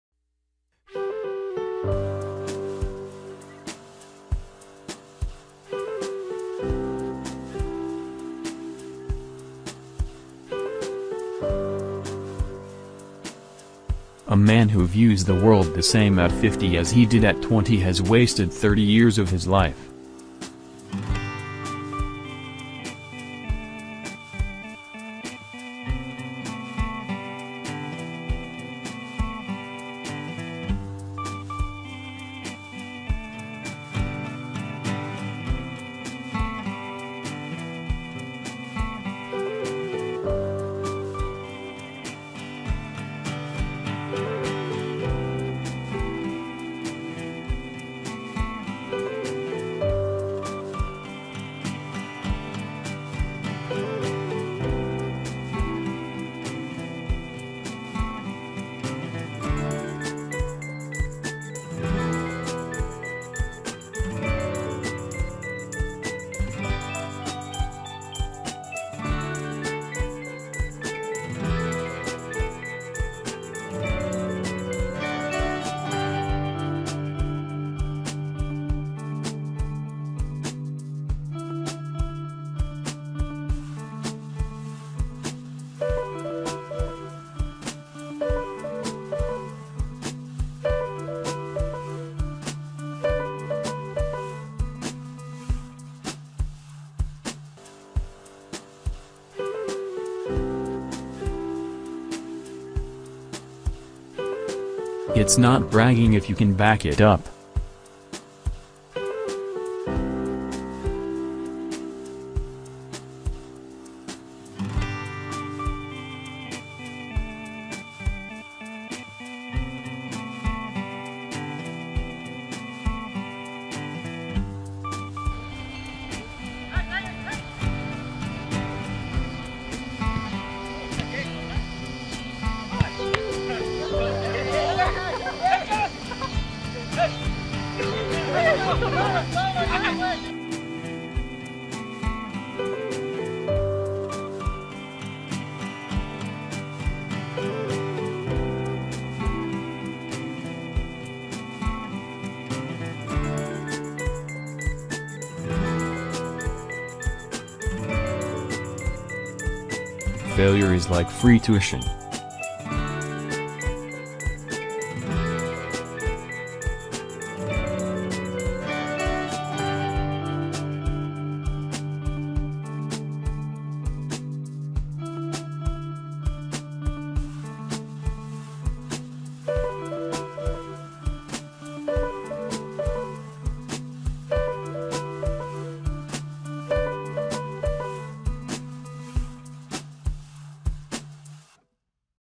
Sporting motivation
Motivation is the foundation of all athletic effort and accomplishment. Without your desire and determination to improve your sports performances, all of the other mental factors, confidence, intensity, focus, and emotions, are meaningless. To become the best athlete you can be, you must be motivated to do what it takes to maximize your ability and achieve your goals.